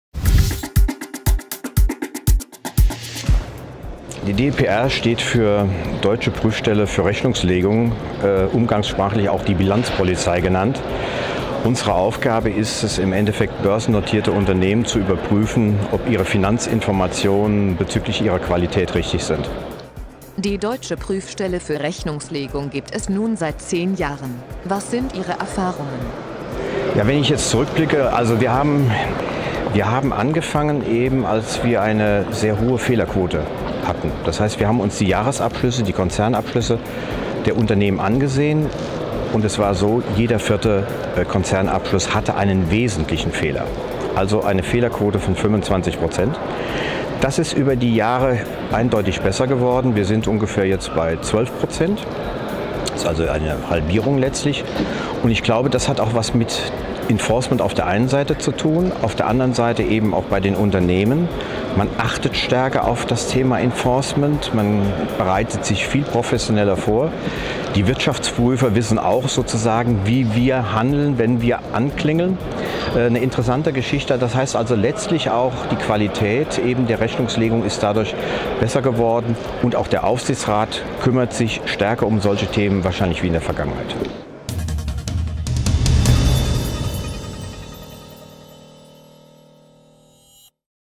DPR: Die Bilanzpolizei?
Prof. Dr. Edgar Ernst über die ersten 10 Jahre der deutschen Prüfstelle für Rechnungslegung.

Zum Nachlesen: Rede von Prof. Ernst anlässlich "10 Jahre DPR" mit folgenden Inhalten: